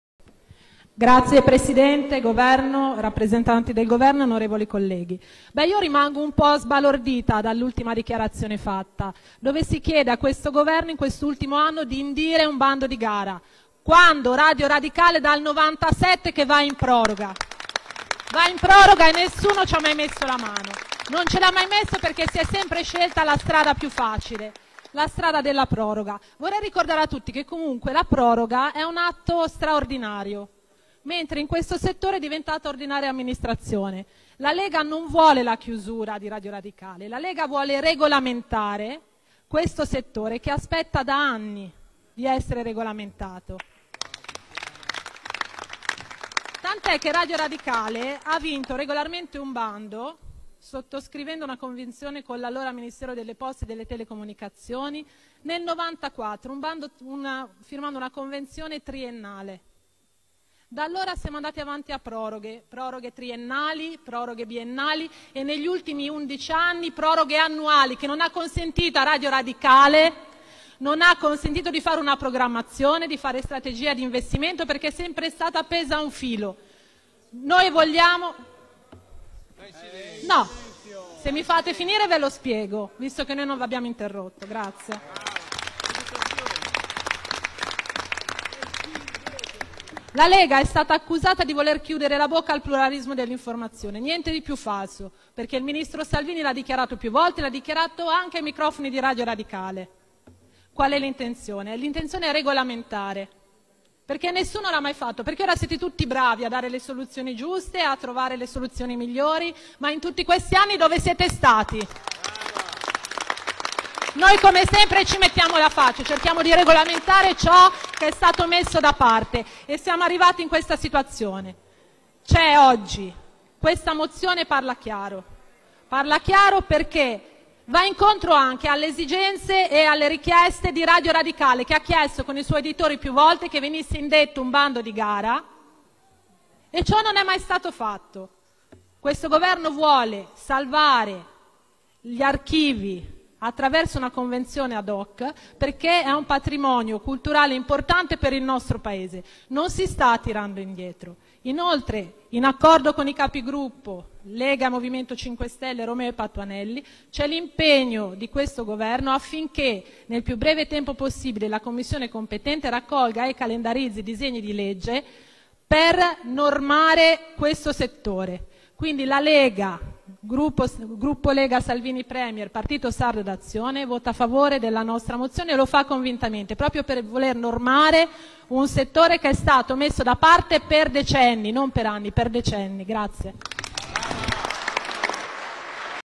Tiziana Nisini su radio radicale
Intervento della senatrice della Lega Tiziana Nisini sulla questione di radio radicale